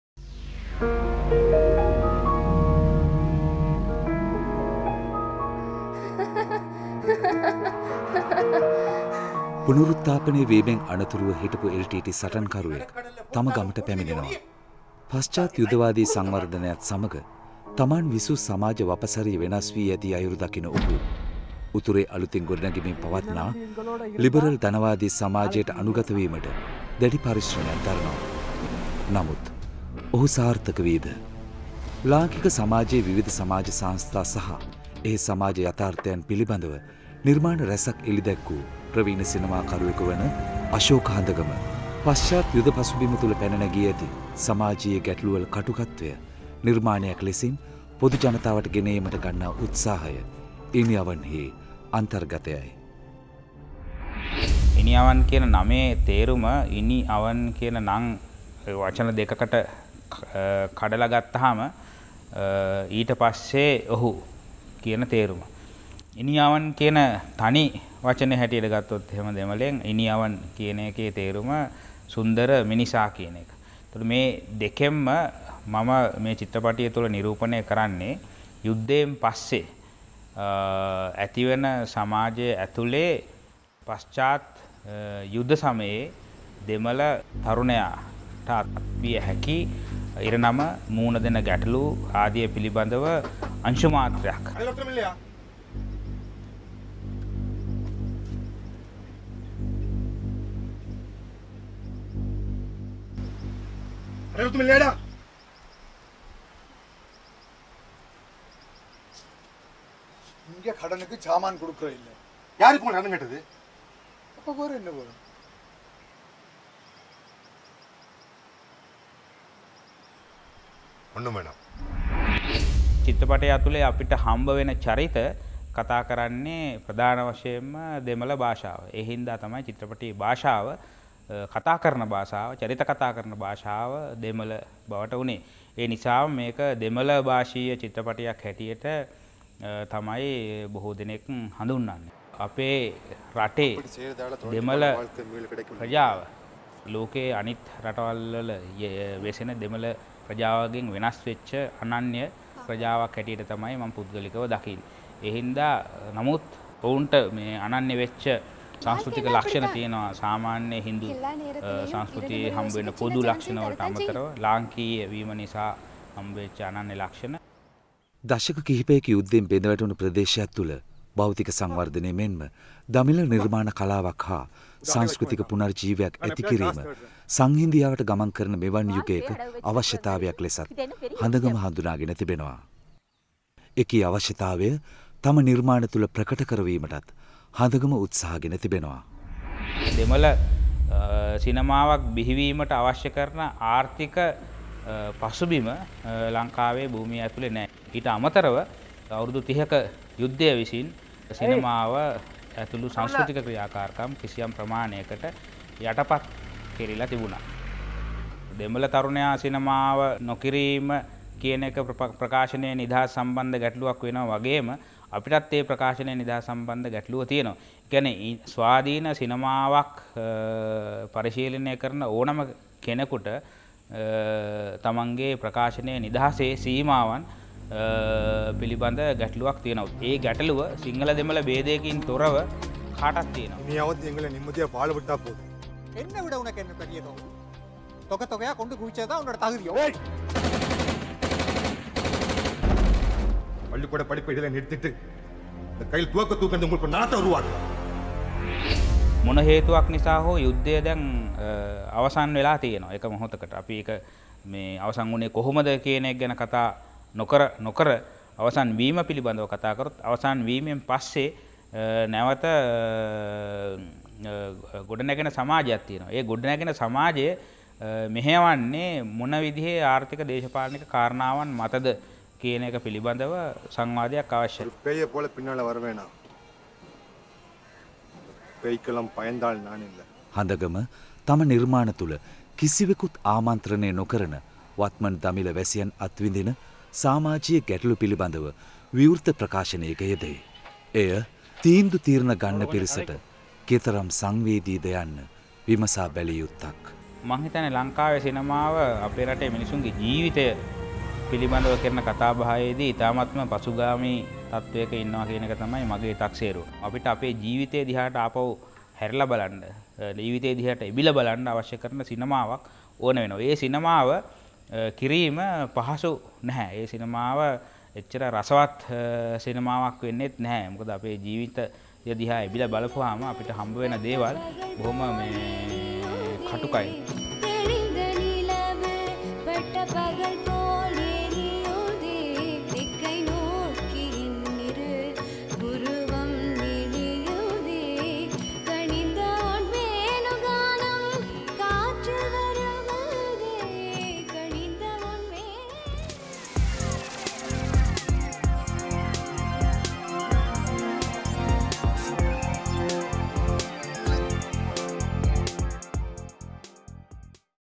හඳගම සමඟ ඉනිඅවන්....
ඉනීඅවන්. අශෝක හඳගමගේ අලූත්ම සිනමා නිර්මාණය. බලන්න යන්න කලින් ප්‍රේක්ෂක ඔබට ඒ පිළිබඳව මේ කෙටි පුර්විකාව.